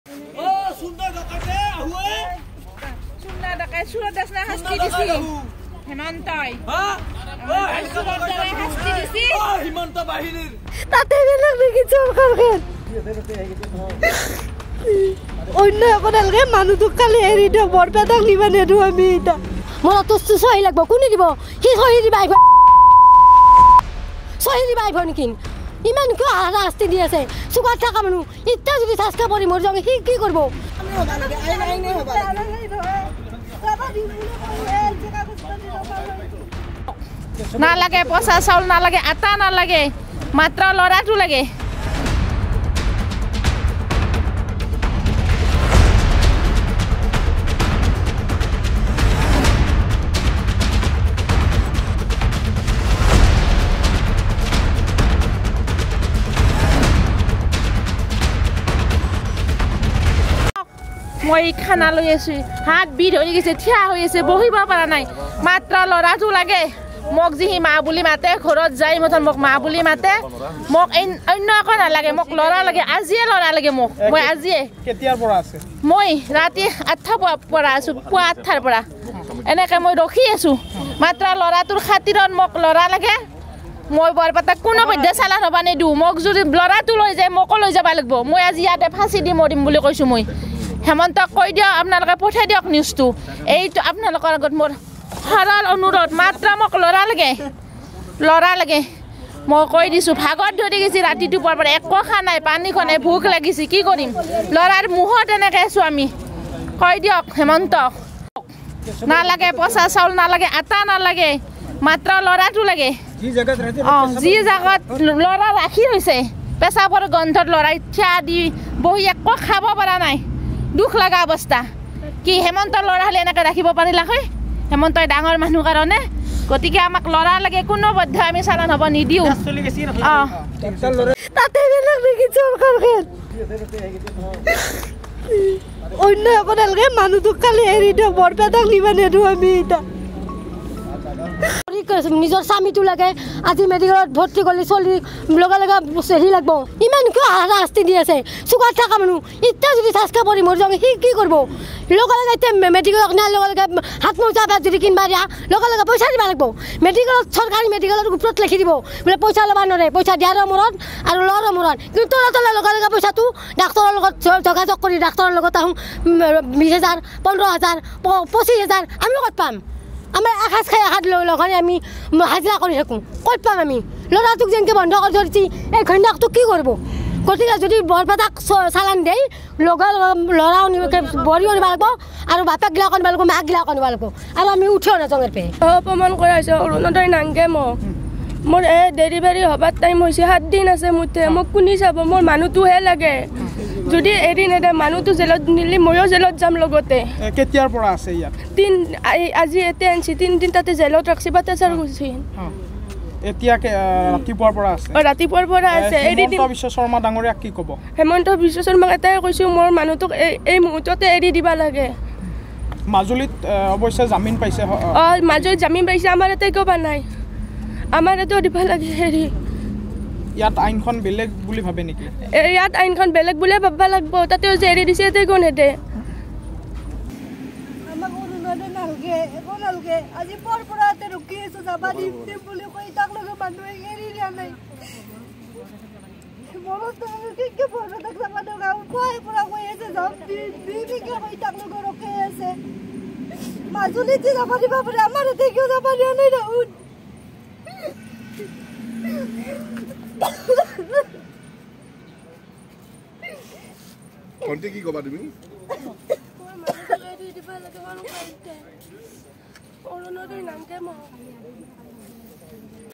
‘অন্য একো নালাগে, মোৰ মানুহটোক এৰি দিয়ক’, ‘অৰুণোদয় নালাগে, ডেলিভাৰী হ’বলৈ ৭ দিন আছে মোক কোনে চাব ?’
‘অন্য একো নালাগে, মোৰ মানুহটোক এৰি দিয়ক’
‘আমাক পঁচা চাউল, আটা নালাগে, ল’ৰাটো ঘূৰাই দিয়ক’
‘অৰুণোদয় নালাগে মোক, ডেলিভাৰী হ’বলৈ ৭ দিন আছে মোক কোনে চাব ?’
স্বামীৰ মুকলিৰ দাবীত কান্দোনত ভাগি পৰিল অন্তঃসত্ত্বা পত্নী